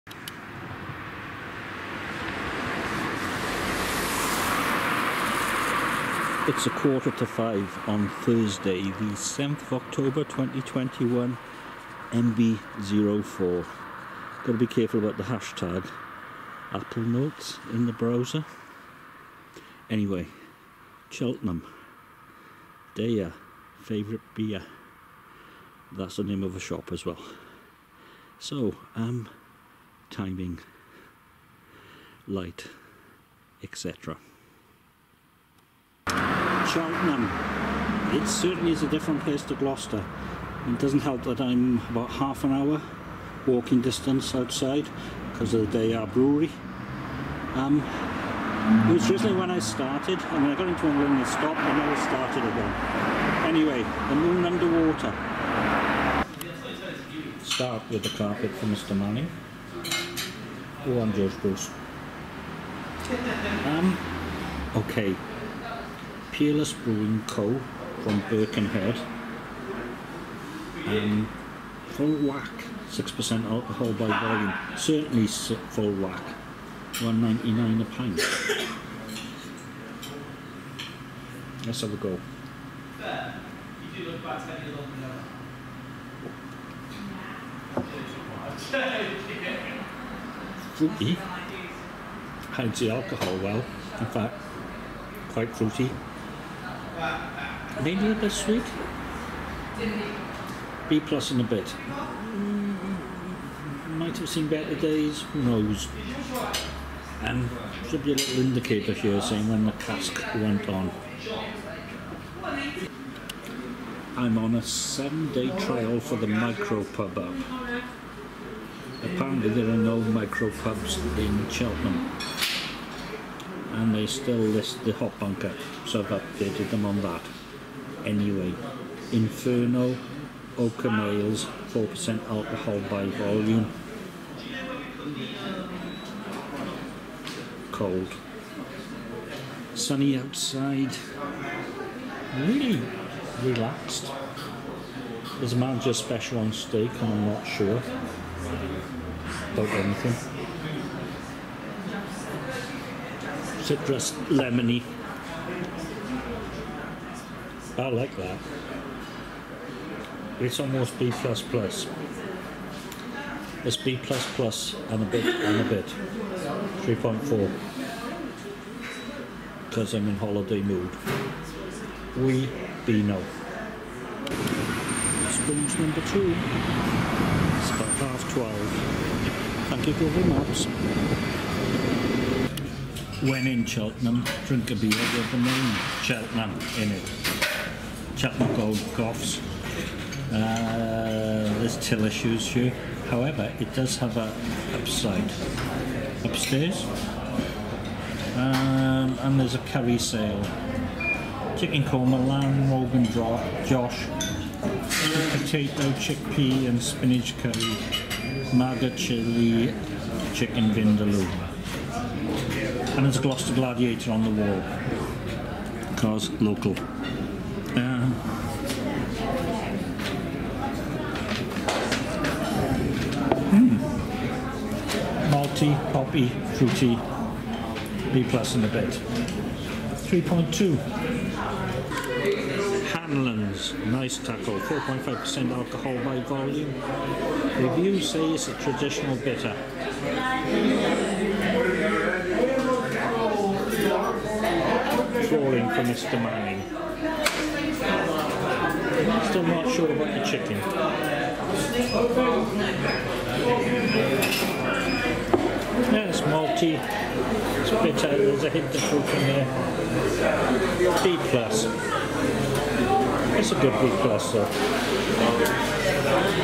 Cheltenham Newbie
Optimised for light departure doesn’t always work on a weekday and then you hit the rush hour, however I think I did the best I could do. 35 minutes from the Premier Inn to Spoons. Again the best I could do ie in the middle between town and Deya. Really like The Moon Under Water and iPad Mini 6 is running like the wind. No Jelly scrolling.

The Bank House, two floors and the smell of Curry Thursday. Of the two Spoons, The Moon Under Water wins.